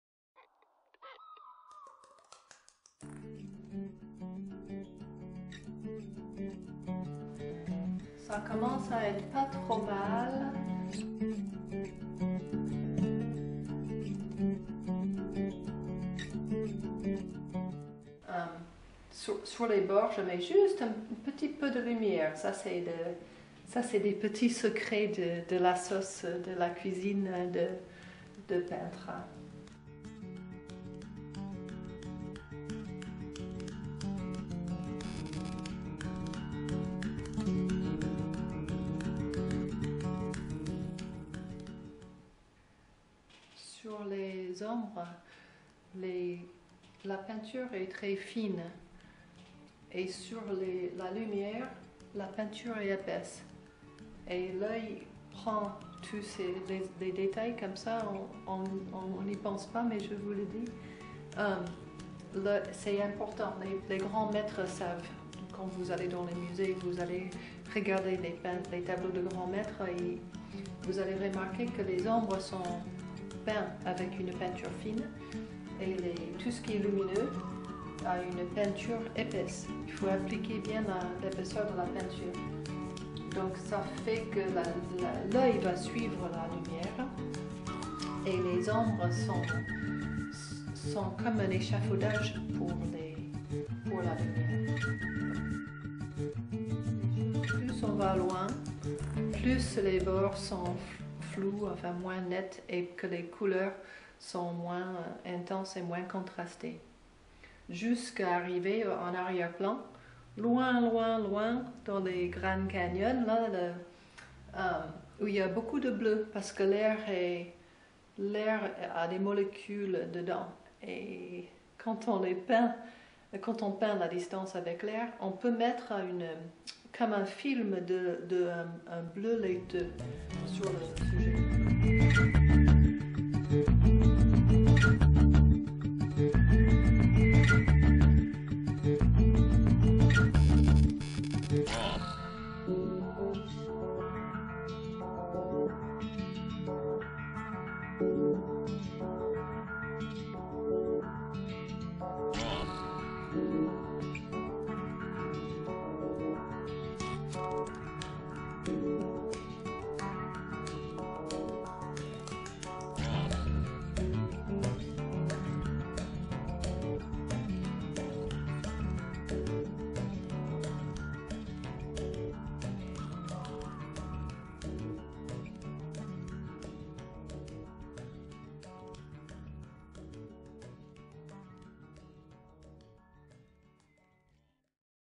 les lys
Artiste peintre, Leslie Varela, parle d'une de ses toiles, 'Lillies and tea cups', avec une démonstration à l'huile.